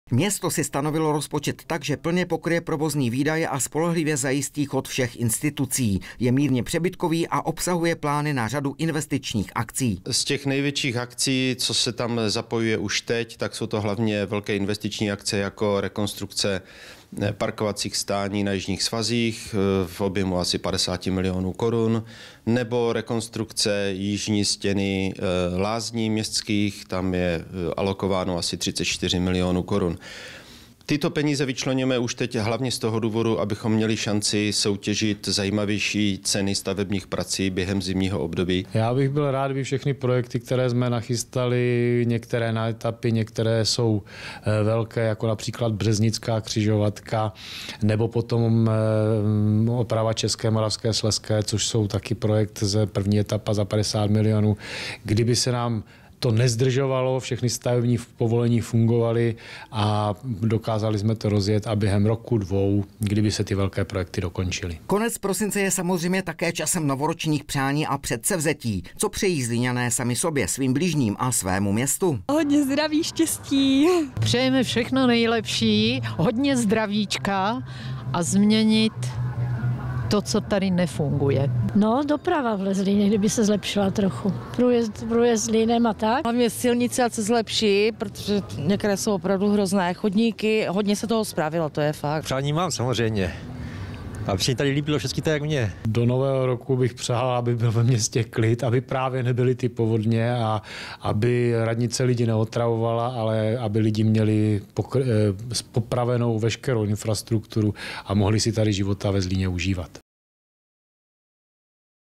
ZLÍN - Výhled na rok 2018
Ve středu 20. prosince začal na zlínské radnici už doopravdy vánoční čas posledním letošním setkáním vedení města s novináři. Závěr roku je vždy časem bilancování, ale stejně tak i výhledů do budoucnosti. Primátora jsme se zeptali na nejdůležitější položky rozpočtu na příští rok, včetně nejdůležitějších investic. V předvánočně naladěných ulicích jsme zase sondovali přání a očekávání občanů.